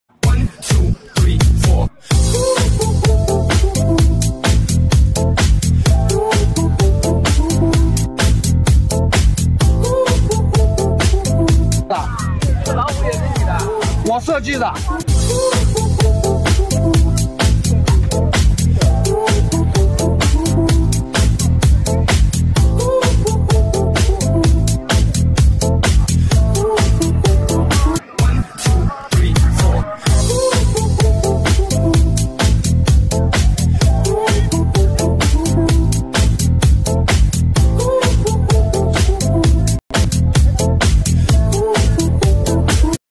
What are the “big guys” at the Weifang International Kite Festival in Shandong?
More than 600 kite lovers from 59 countries and regions gathered in Weifang, Let's see what “big guys” are soaring in the sky this year.